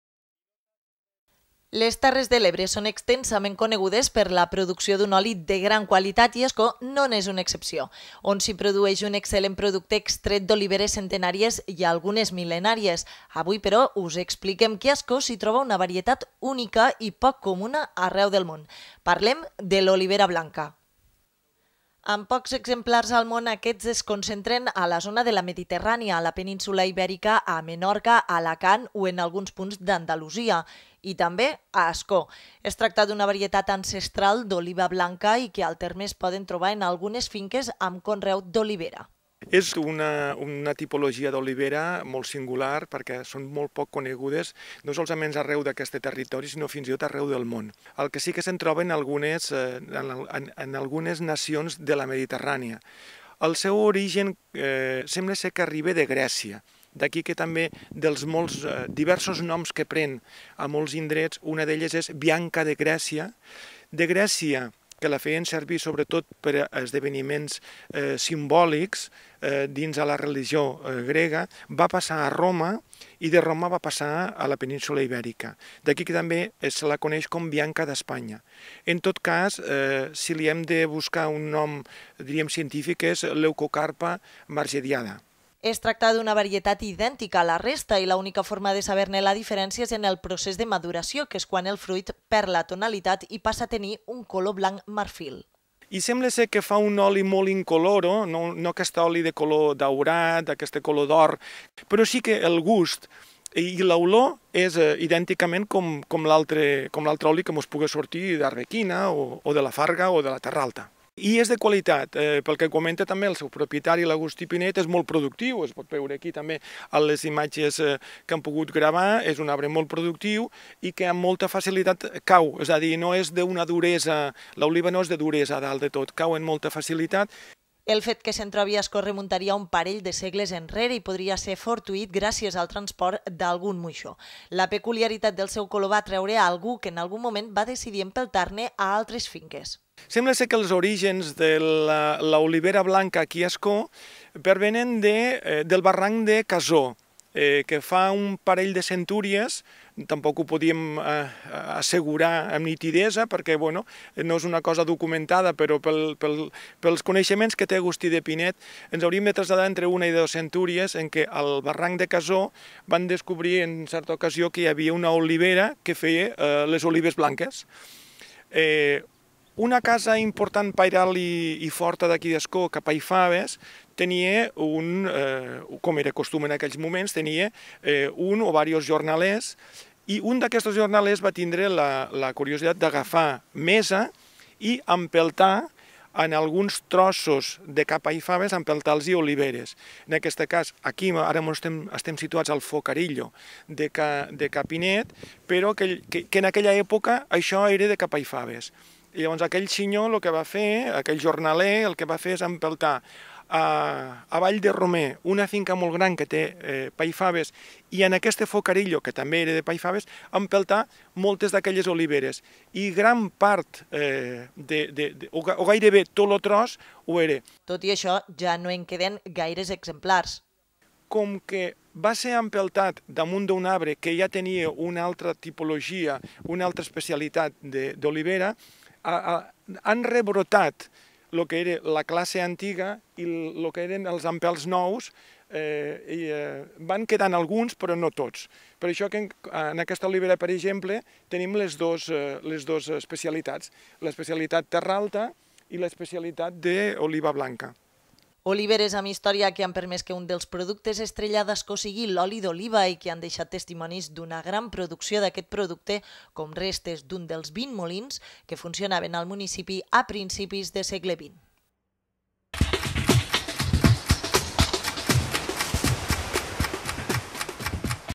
Ascó Notícies: L'oliva blanca, una varietat poc comuna al món, es pot trobar a Ascó.
21/12/21.- Les terres de l’Ebre són extensament conegudes per la producción d’un oli de gran qualitat, i Ascó no n’és una excepció, on s’hi produeix un excel·lent producte extret d’oliveres centenàries i algunes mil·lenàries. Avui però, us expliquem que a Ascó s’hi troba una varietat única i poc comuna arreu del món. Parlem de l’oliva blanca.